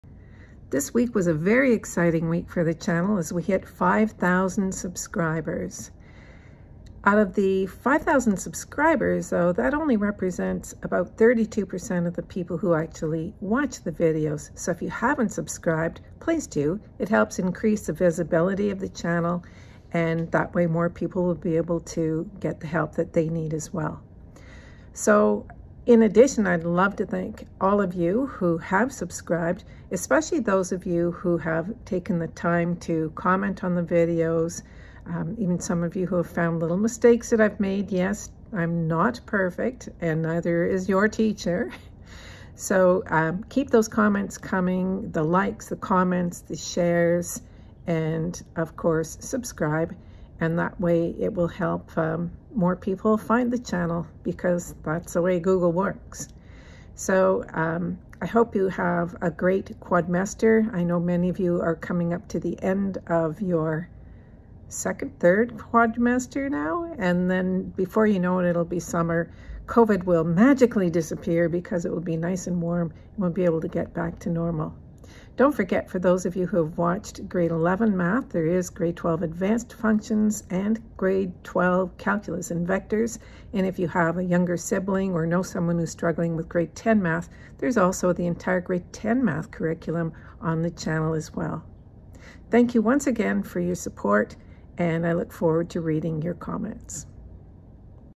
5000 + subscribers!
A BIG thank you to all my subscribers who have helped the channel grow.
When I began the channel in September of 2018  I knew that it would be helpful for many students who couldn't afford a tutor but I obviously had no idea how very important it would become in 2020/2021.
Many thanks to all of you who like, share and have subscribed and also to those of you who have taken the time to thank me and post such amazing heart warming comments.
Ms H